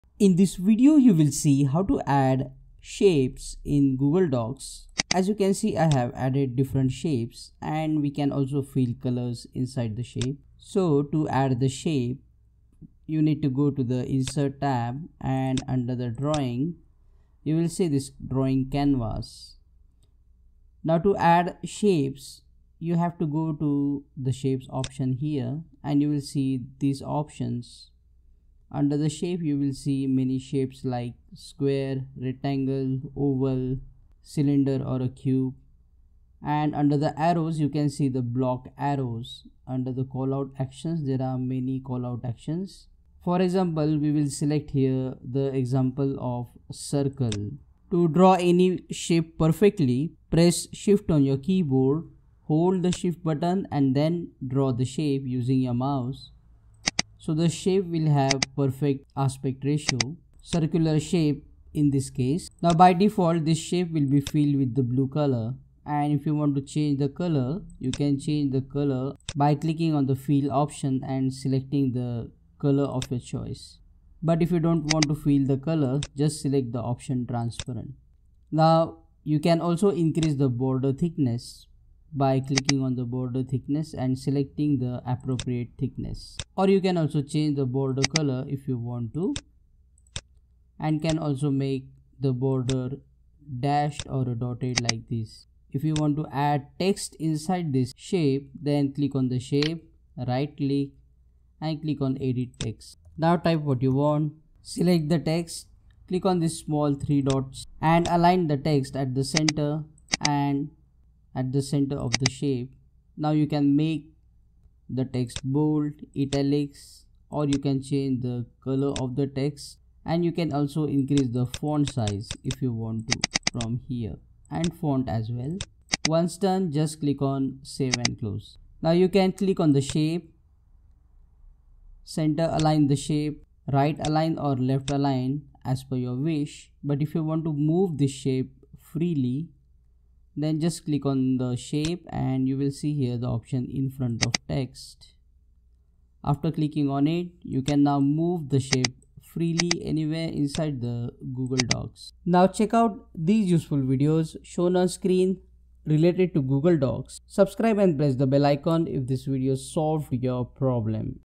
How To Add Shapes In Google Docs
Watch in this video How To Add Shapes In Google Docs document like circle square, rectangle or blocked arrows. Also, see How to move shapes in Google Docs freely